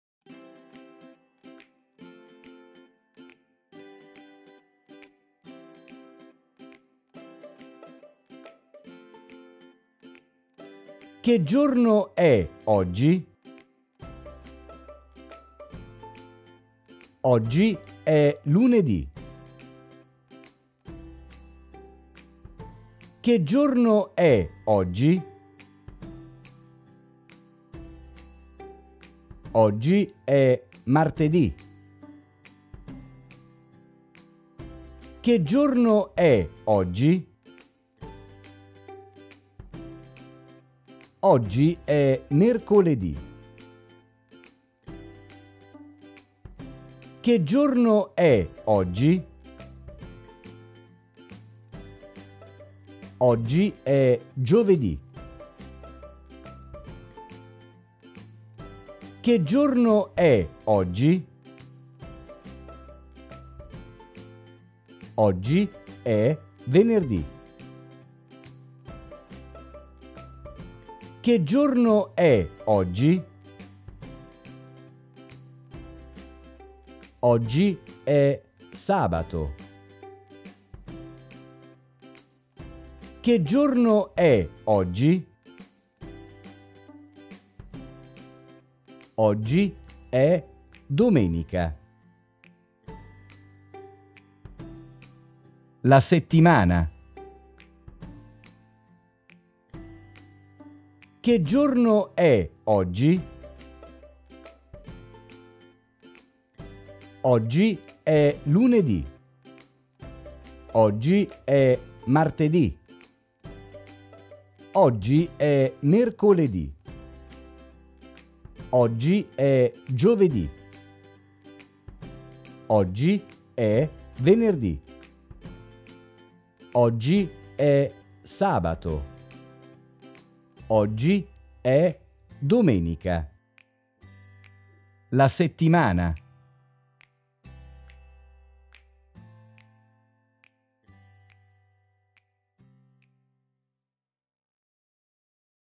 What's the day today? - Italian for children - "Che giorno e oggi?"
Practise the week days in Italian with our fun video. Don't forget to repeat the words!